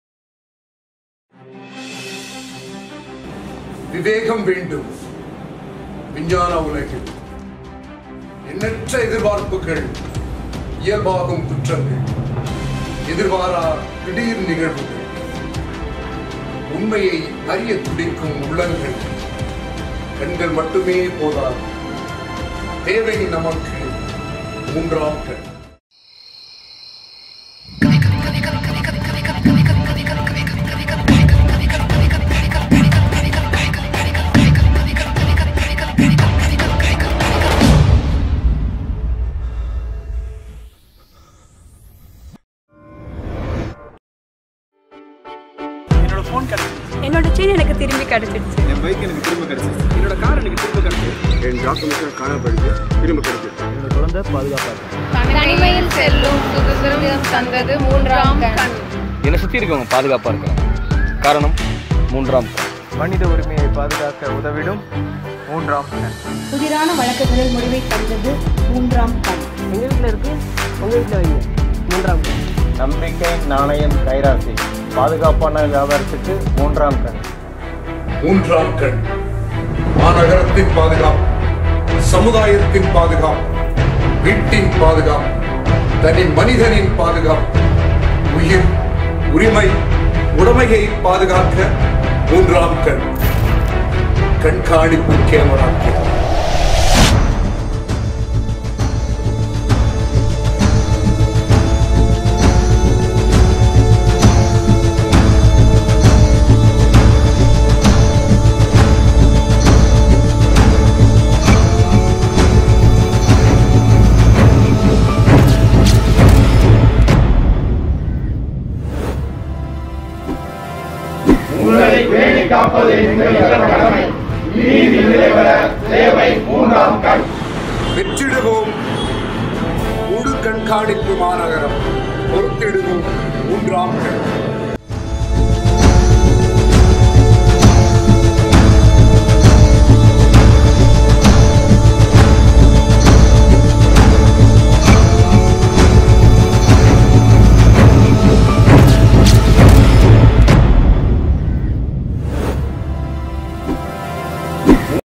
CCTV Awareness 2
CCTV camera surveillance in Chennai. The city police personnel have been conducting door-to-door campaigns, calling on households and commercial establishments on every street, asking them to set up CCTVs.